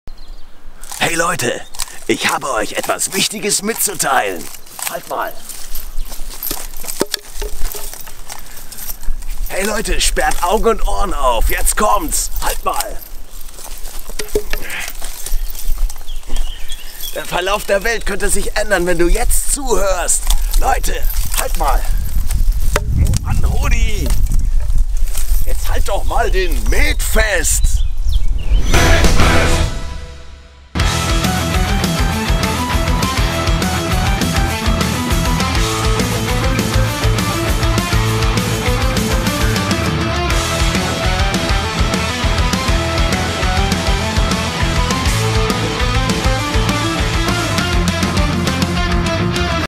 Feuerschwanz - Metfest Tailer 2019
15 Jahre Feuerschwanz! 2019 sind wir auf großer METFEST-Jubiläums-Tour! Ein episches Metgefecht erwartet Euch - lasst uns 15 Jahre Bandgeschichte eine Krone aufsetzen!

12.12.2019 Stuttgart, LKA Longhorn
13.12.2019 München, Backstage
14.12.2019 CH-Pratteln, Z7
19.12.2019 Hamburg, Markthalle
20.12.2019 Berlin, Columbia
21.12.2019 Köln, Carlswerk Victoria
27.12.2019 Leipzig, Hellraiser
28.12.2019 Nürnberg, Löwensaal

Support: Grailknights + The Paddyhats!